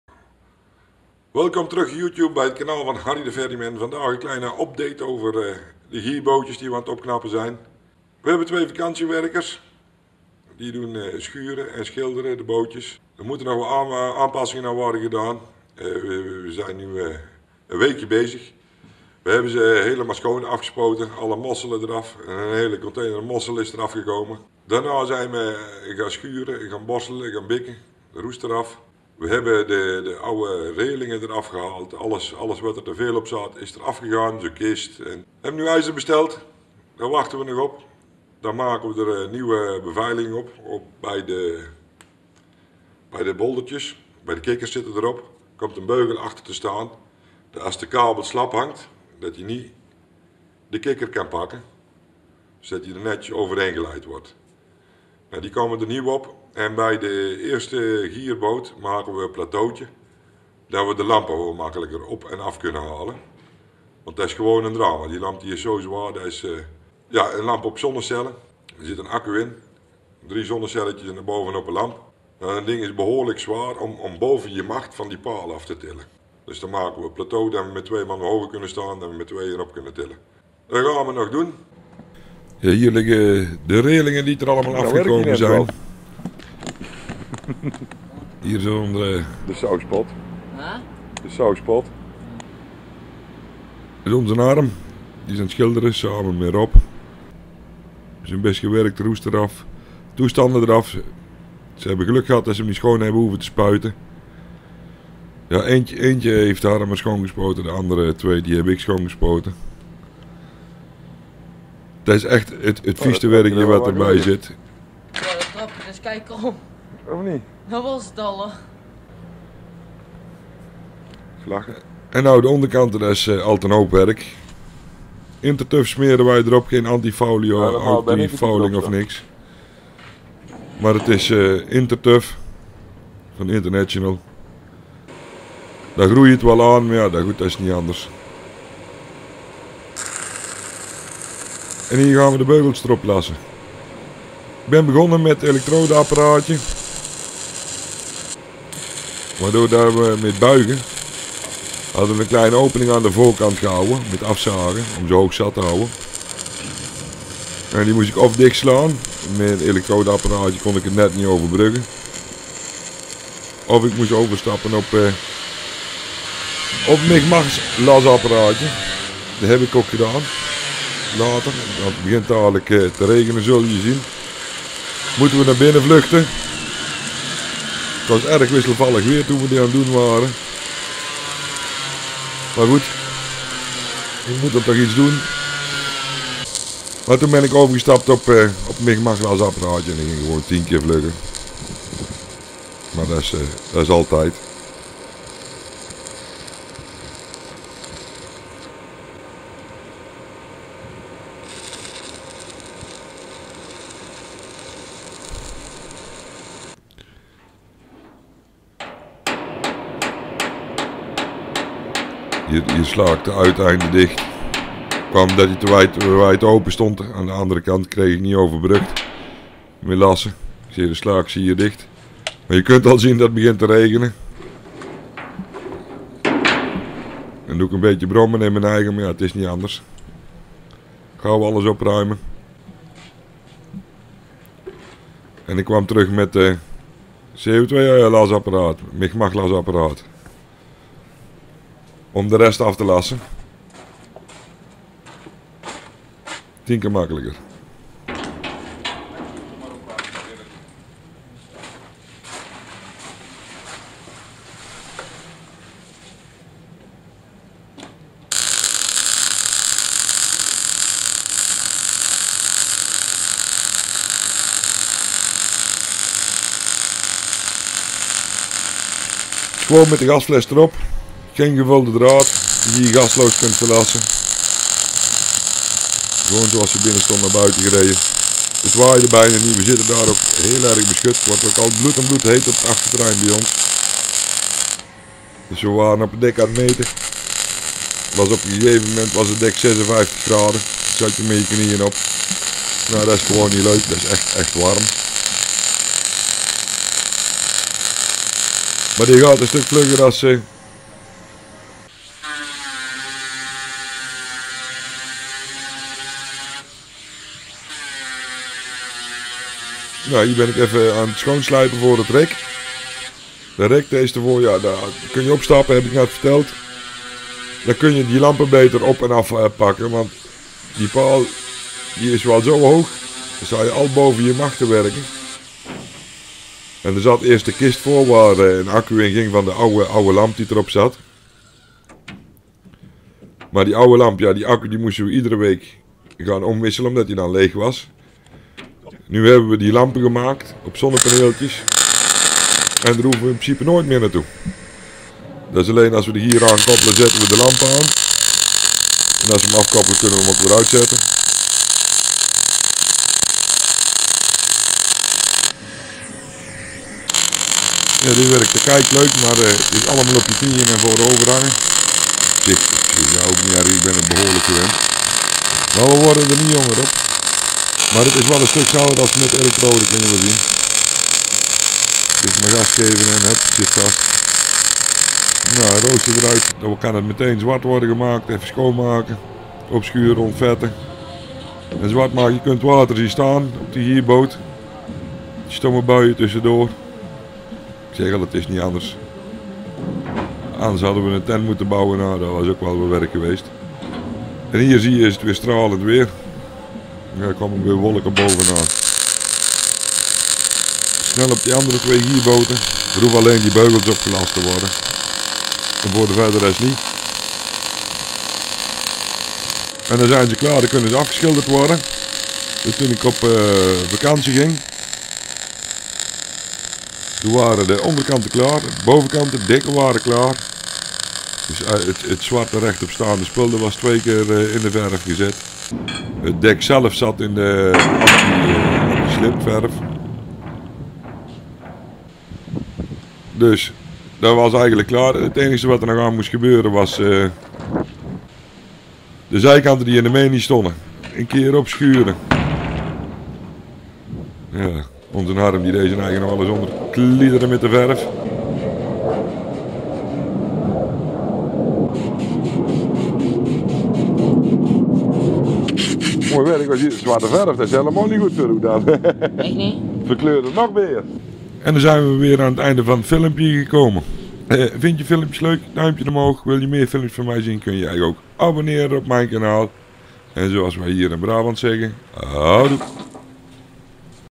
De gierboten van Veerpont Alphen aanpassen en schilderen!!!
Net voor de vakantie nog de beugels op de boten gelast en het rek er op gelast. Met het lassen zijn we klaar alleen het geel moet nog geschilderd worden en dan kunnen ze terug naar Alphen.